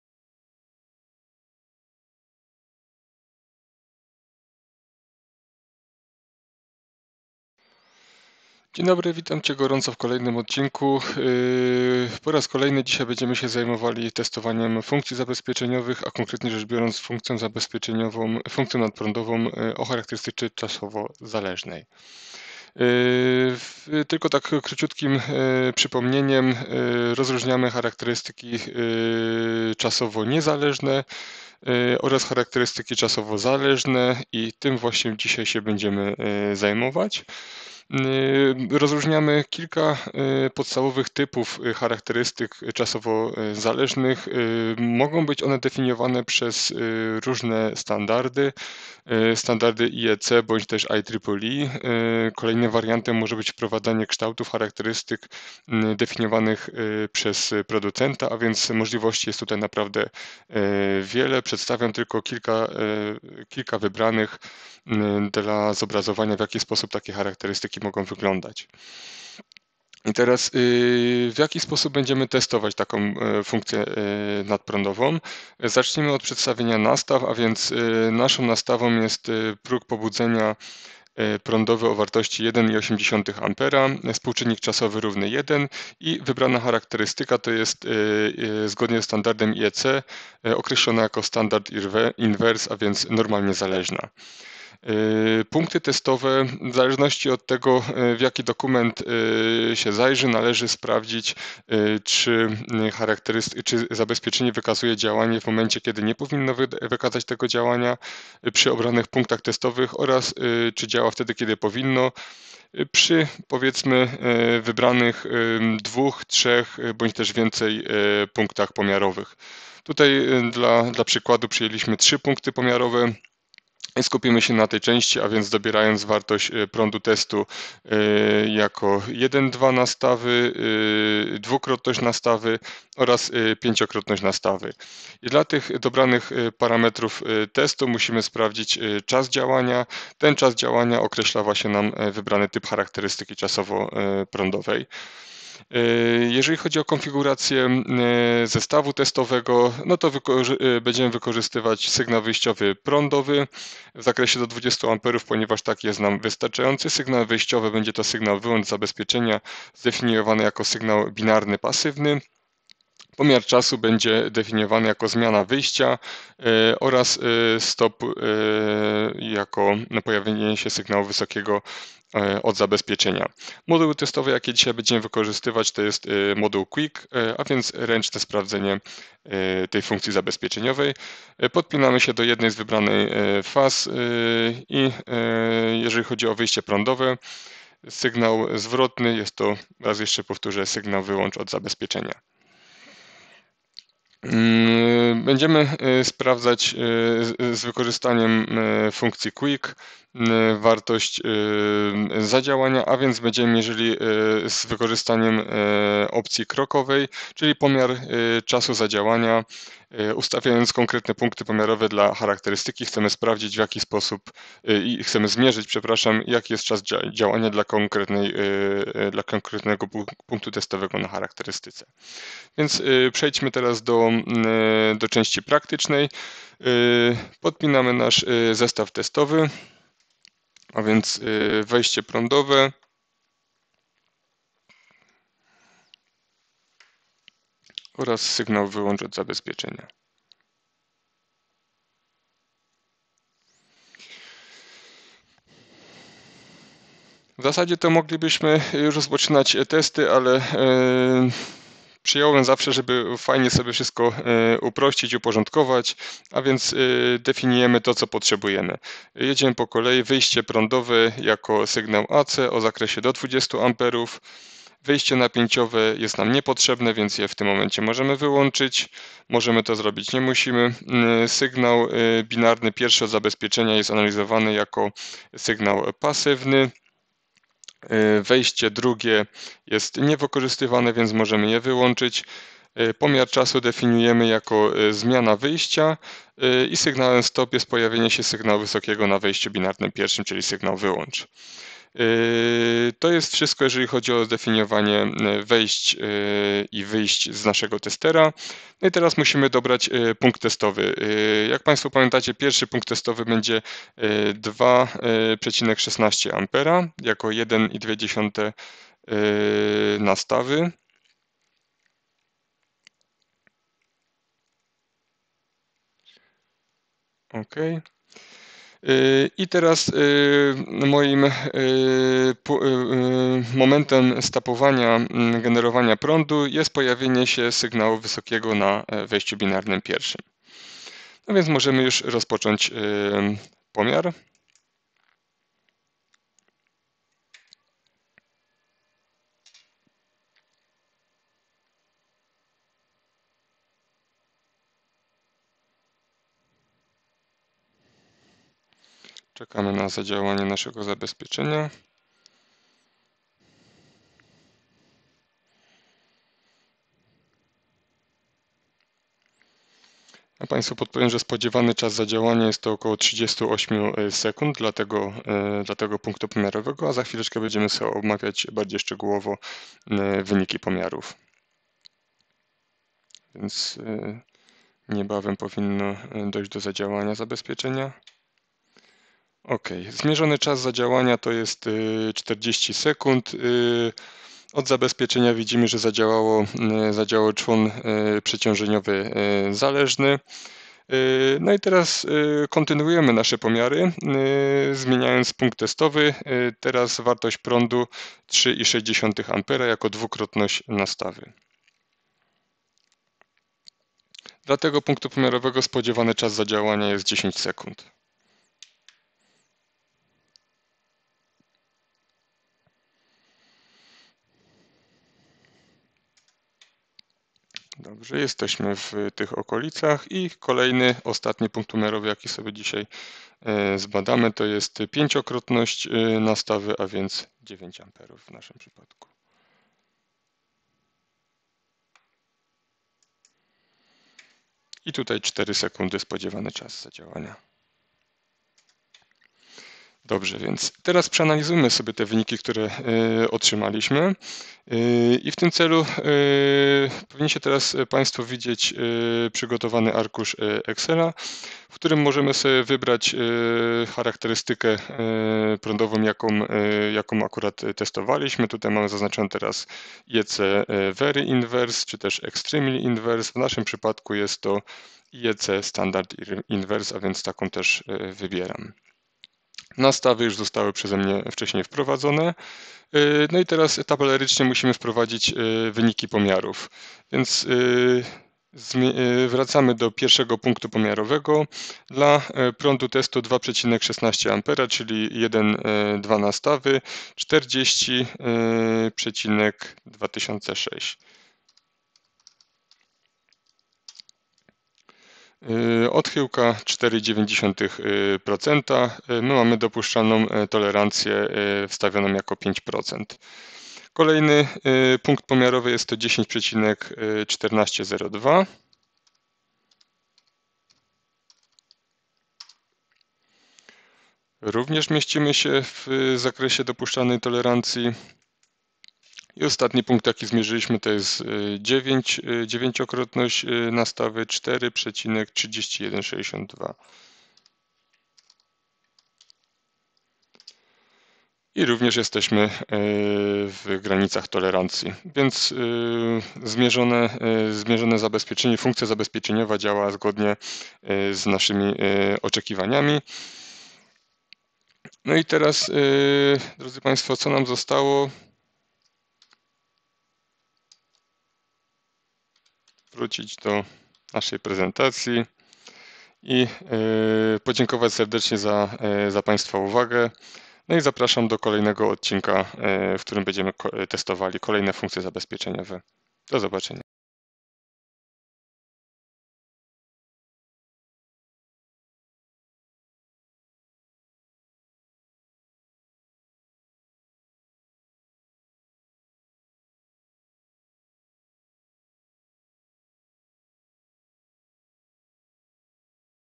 Badanie zabezpieczenia nadprądowego o charakterystyce czasowo-zależnej
W tym odcinku znajdziesz informację na temat badania zabezpieczenia nadprądowego o charakterystyce czasowo-zależnej:

- dobór parametrów testu (czas zadziałania),
- konfiguracja automatycznego trybu testu,
- konfiguracja sprzętowa urządzenia testowego COMPANO 100,
- ocena.

Całość omówionych teoretycznie zagadnień poparta jest praktycznymi testami z wykorzystaniem cyfrowego terminala zabezpieczenia.